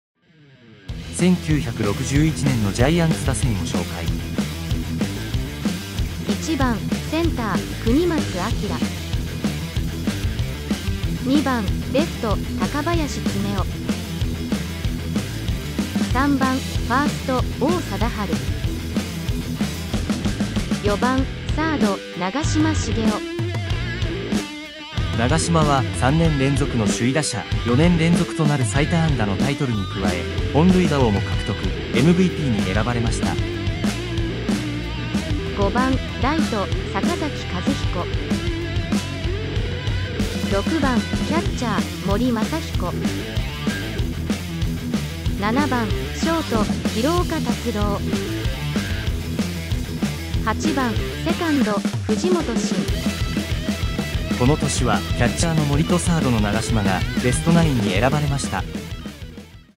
1961年の巨人打線―長嶋茂雄が本塁打、打率の2冠でMVPを獲得！【読売ジャイアンツの歴史 昭和36年】
1961年のジャイアンツは、川上哲治監督の元、リーグ優勝、日本シリーズ制覇を達成しました。MVPは不動の4番、長嶋茂雄。
長嶋は、4年連続の最多安打、3年連続の首位打者に、ホームラン王のタイトルも獲得しました。